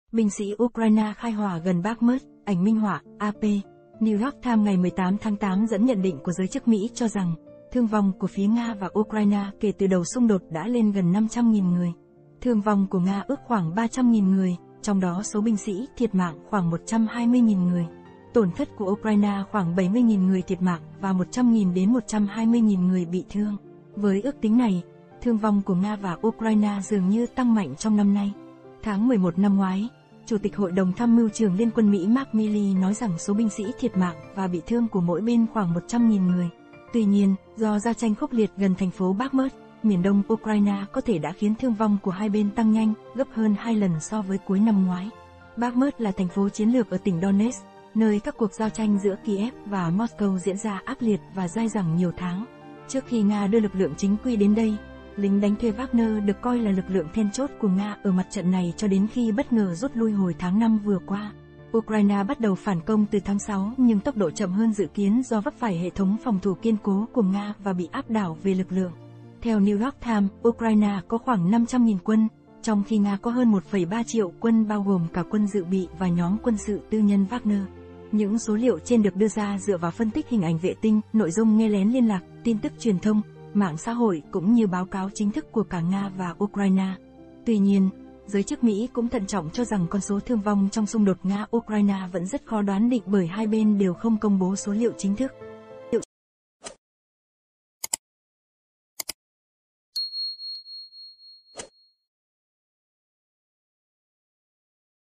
THƯƠNG VONG TĂNG CAO TRONG XUNG ĐỘT NGA - UKRAINE
Binh sĩ Ukraine khai hỏa gần Bakhmut (Ảnh minh họa: AP). New York Times ngày 18/8 dẫn nhận định của giới chức Mỹ cho rằng, thương vong của phía Nga và Ukraine kể từ đầu xung đột đã lên gần 500.000 người. Thương vong của Nga ước khoảng 300.000 người, trong đó số binh sĩ thiệt mạng khoảng 120.000 người. Tổn thất của Ukraine khoảng 70.000 người thiệt mạng và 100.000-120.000 người bị thương. Với ước tính này, thương vong của Nga và Ukraine dường như tăng mạnh trong năm nay. Tháng 11 năm ngoái, Chủ tịch Hội đồng Tham mưu trưởng Liên quân Mỹ Mark Milley nói rằng số binh sĩ thiệt mạng và bị thương của mỗi bên khoảng 100.000 người. Tuy nhiên, do giao tranh khốc liệt gần thành phố Bakhmut, miền Đông Ukraine có thể đã khiến thương vong của hai bên tăng nhanh, gấp hơn 2 lần so với cuối năm ngoái. Bakhmut là thành phố chiến lược ở tỉnh Donetsk, nơi các cuộc giao tranh giữa Kiev và Moscow diễn ra ác liệt và dai dẳng nhiều tháng. Trước khi Nga đưa lực lượng chính quy đến đây, lính đánh thuê Wagner được coi là lực lượng then chốt của Nga ở mặt trận này cho đến khi bất ngờ rút lui hồi tháng 5 vừa qua. Ukraine bắt đầu phản công từ tháng 6 nhưng tốc độ chậm hơn dự kiến do vấp phải hệ thống phòng thủ kiên cố của Nga và bị áp đảo về lực lượng. Theo New York Times, Ukraine có khoảng 500.000 quân, trong khi Nga có hơn 1,3 triệu quân bao gồm cả quân dự bị và nhóm quân sự tư nhân Wagner. Những số liệu trên được đưa ra dựa vào phân tích hình ảnh vệ tinh, nội dung nghe lén liên lạc, tin tức truyền thông, mạng xã hội cũng như báo cáo chính thức của cả Nga và Ukraine. Tuy nhiên, giới chức Mỹ cũng thận trọng cho rằng con số thương vong trong xung đột Nga - Ukraine vẫn rất khó đoán định bởi hai bên đều không công bố số liệu chính thức.